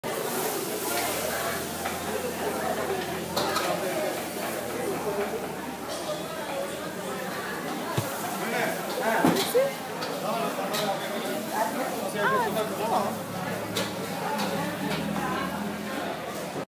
Naan bread being made in Cameron Highlands, Malaysia
Naan Bread is so delicious.